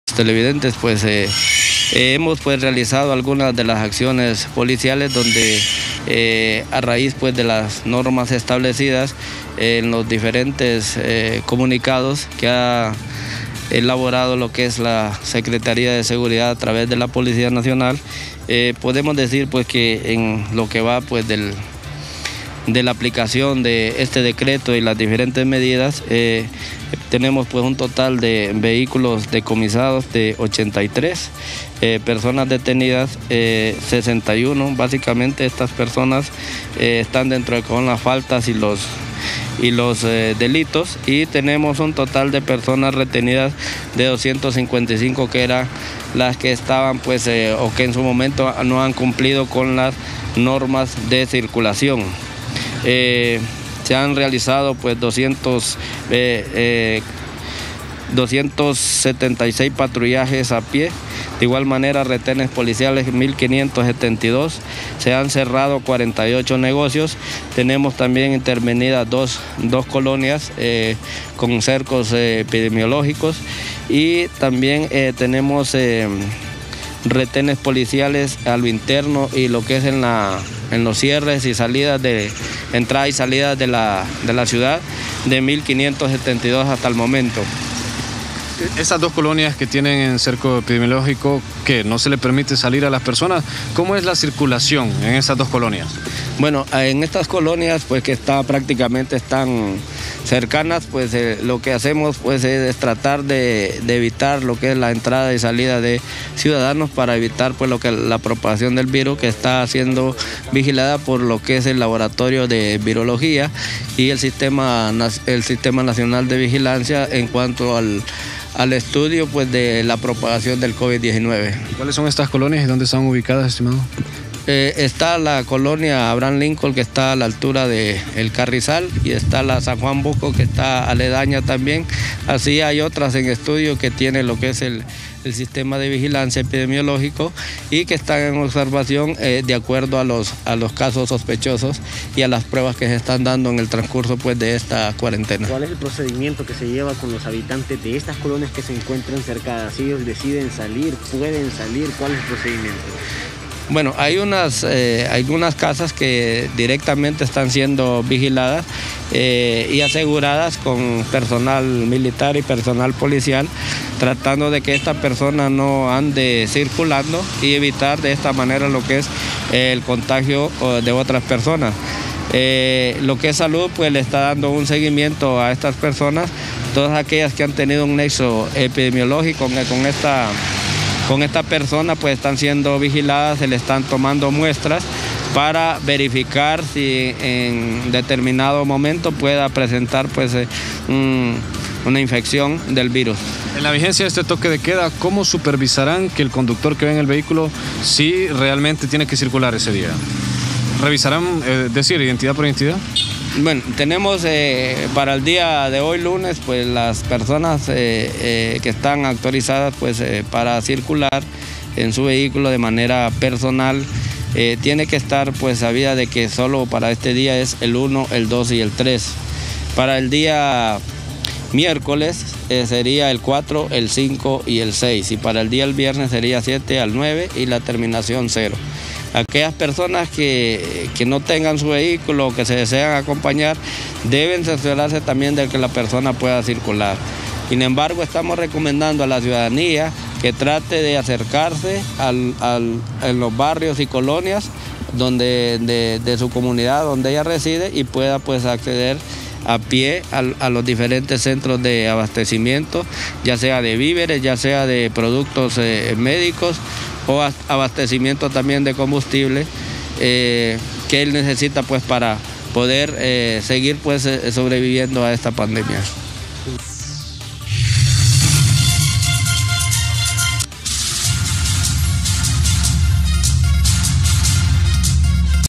83 vehículos decomisados por DNTV por transitar sin salvoconducto
Publicado lunes 30 de marzo del 2020